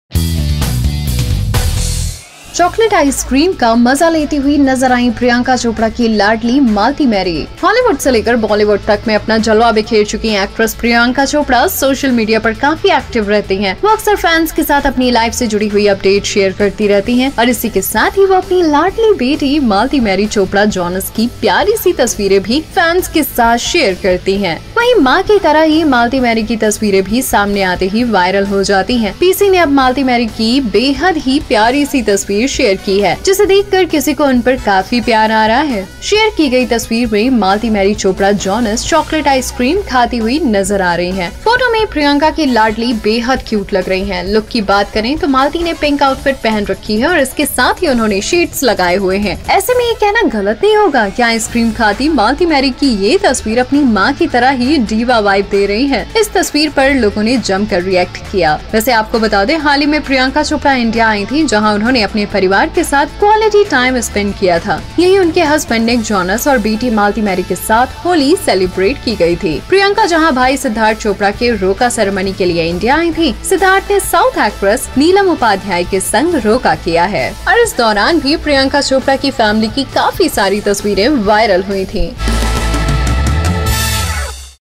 Priyanka Chopra Shares An Adorable Picture Of Malti Enjoying Ice Cream | Priyanka Chopra New Project
Priyanka Chopra shares an adorable picture of Malti enjoying a cone of chocolate ice cream. Priyanka had recently travelled to India with Malti and Nick Jonas. Priyanka resumed work on her ongoing project, 'Heads of State'.